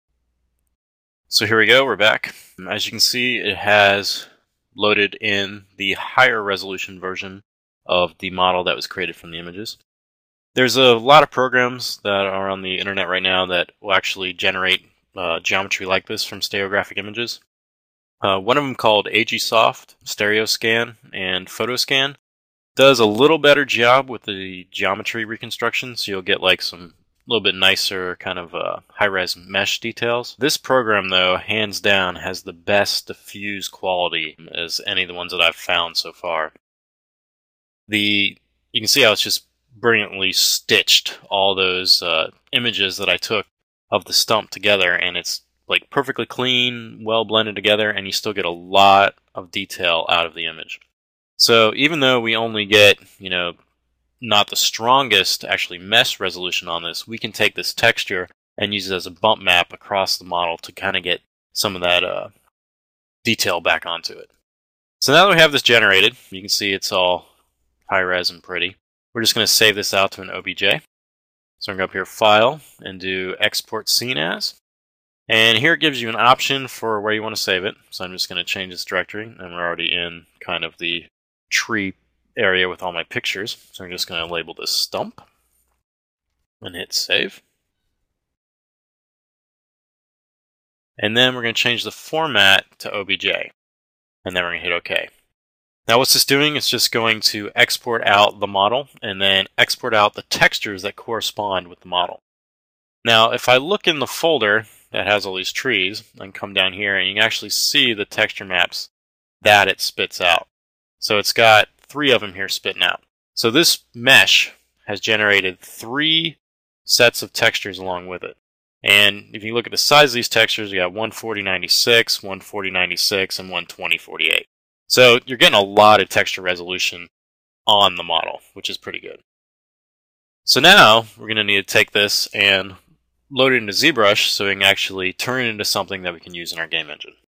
HD: Generating game geometry with simple photographs (Part 2) Joseph Drust
Quick Tutorial by Joseph Drust (Links to other Parts Below)
Using a digital camera, Zbrush 4.0, and Autodesk Photo Scene Editor (photofly) high quality video game geometry can be created quick and easy.